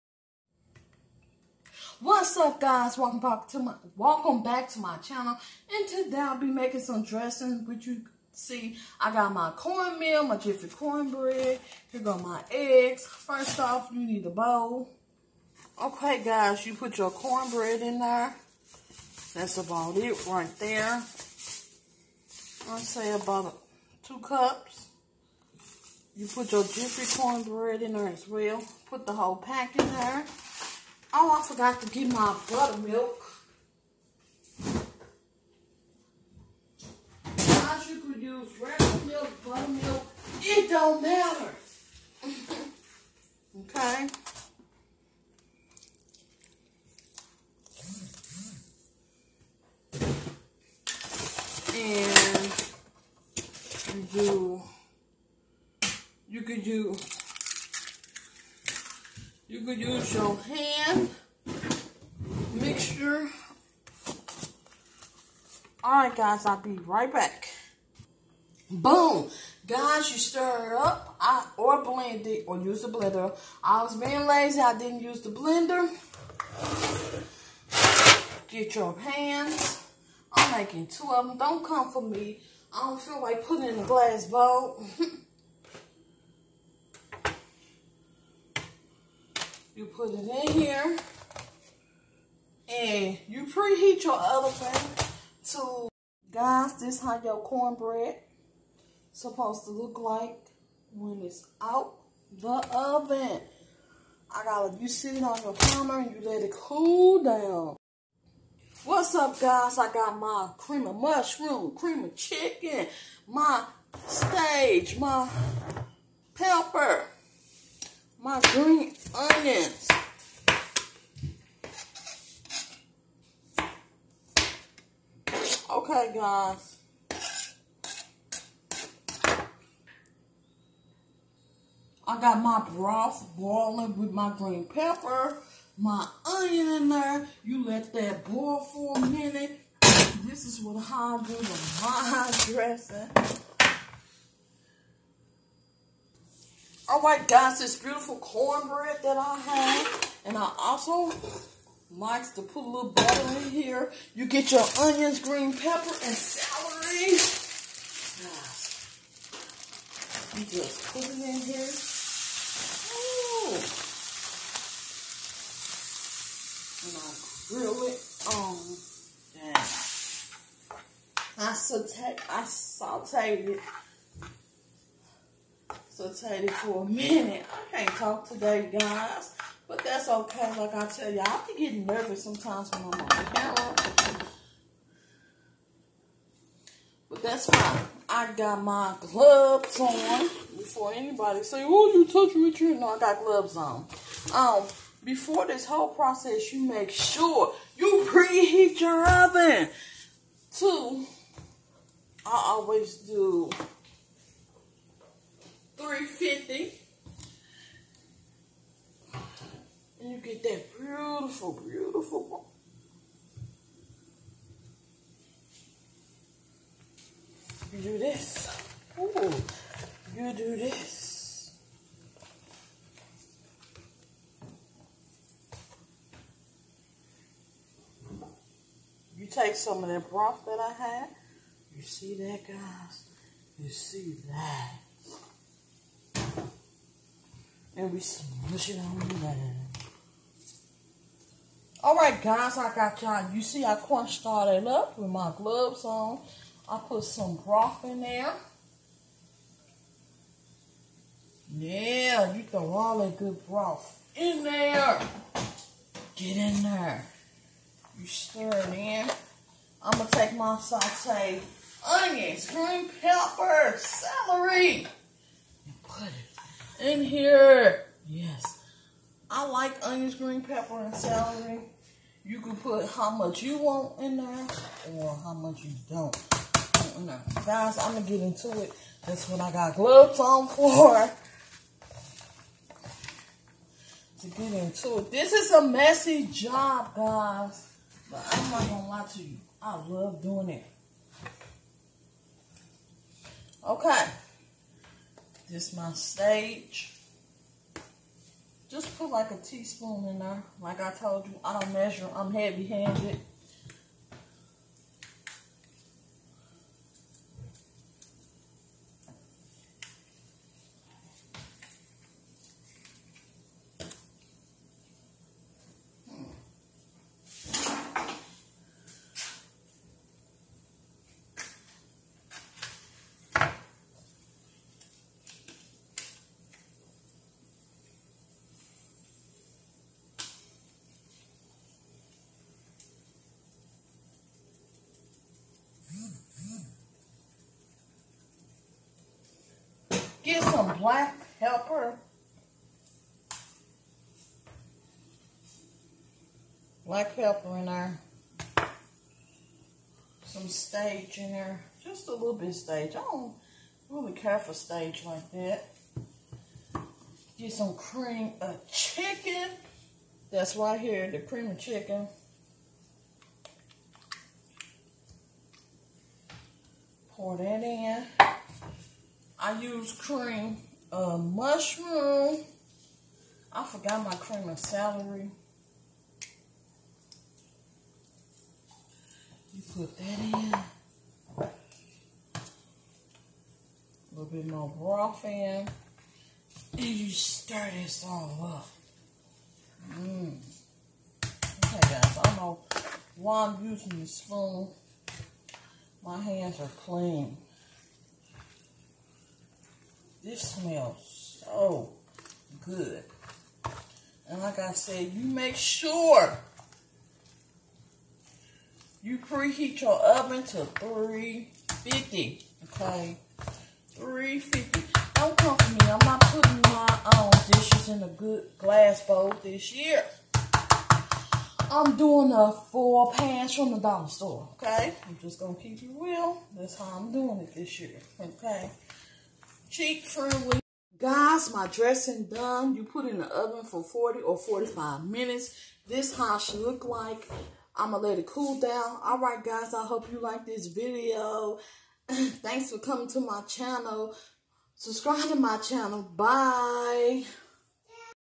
Homemade Dressing
onion green pepper sage black pepper season salt garlic powder cream of chicken soup and cream of mushrooms corn meal and jiffy corn bread mix together and eggs butter milk or regular milk pre heat oven to 350 cook for 40 or 45 minutes depends on how much you make